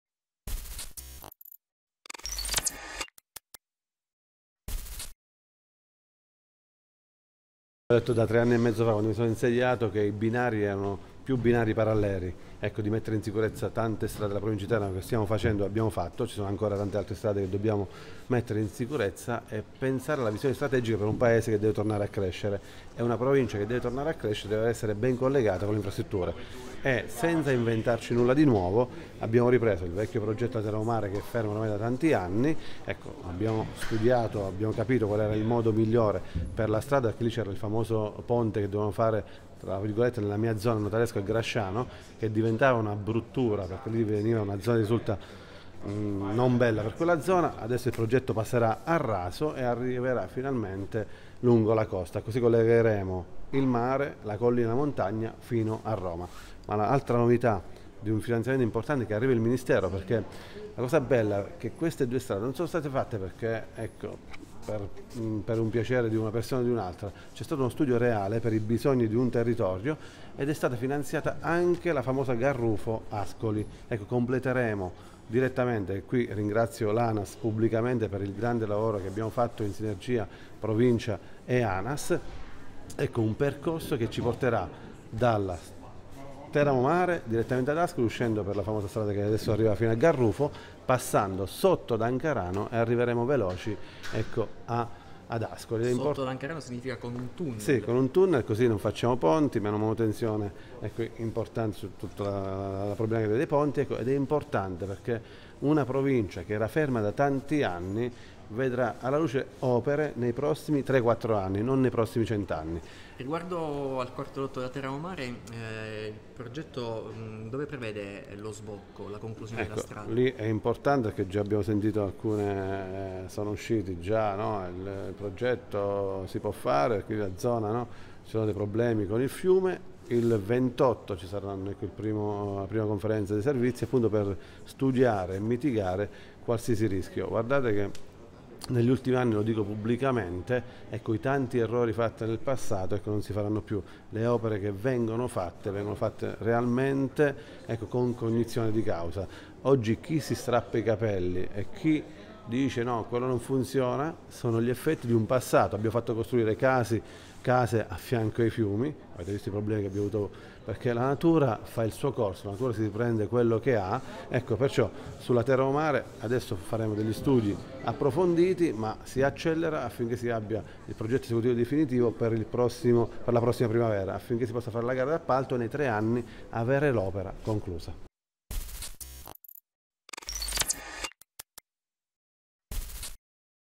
"Teramo-mare e Pedemontana terminate entro il 2026"
Finanziate le opere come annunciato dal presidente della Provincia di Teramo, Diego Di Bonaventura. In primavera, almeno nelle intenzioni, ci sarà la svolta